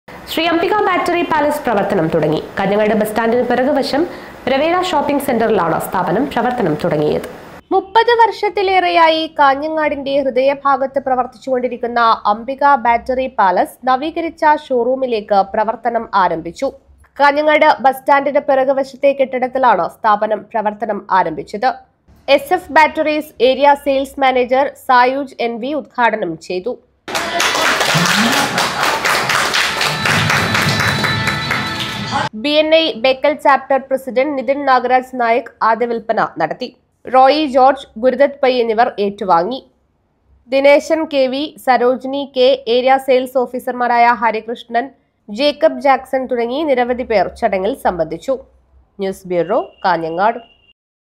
.ശ്രീ അംബിക ബാറ്ററി പാലസ് പ്രവര്‍ത്തനം തുടങ്ങി.
.ശ്രീ അംബിക ബാറ്ററി പാലസ് പ്രവര്‍ത്തനം തുടങ്ങി. കാഞ്ഞങ്ങാട് ബസ്റ്റാന്റിന് പിറക്വശം റെവേര ഷോപ്പിംഗ് സെന്ററിലാണ് സ്ഥാപനം പ്രവര്‍ത്തനം തുടങ്ങിയത്.